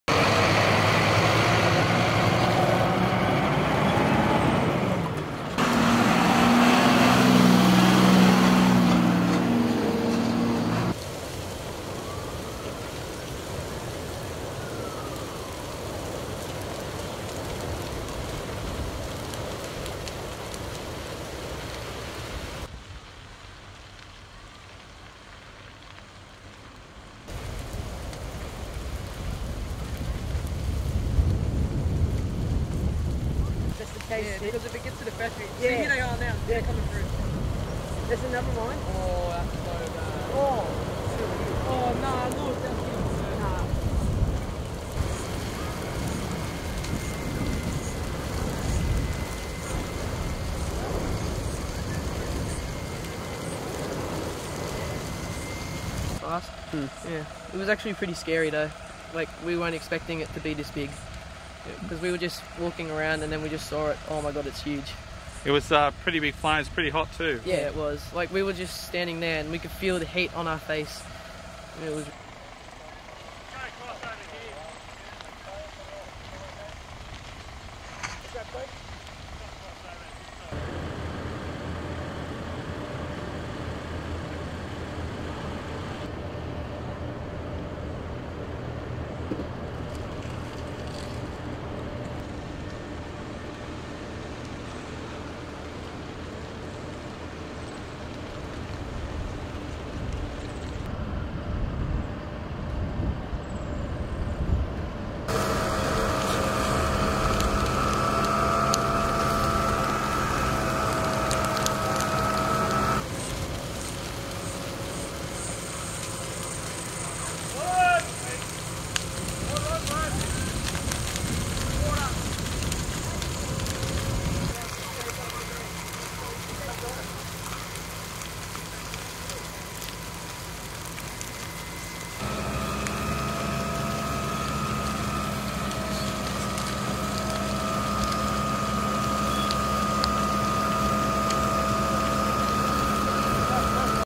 Fire Penrith
Raw footage from a recent fire near Penrith on the outskirts of Sydney Australia.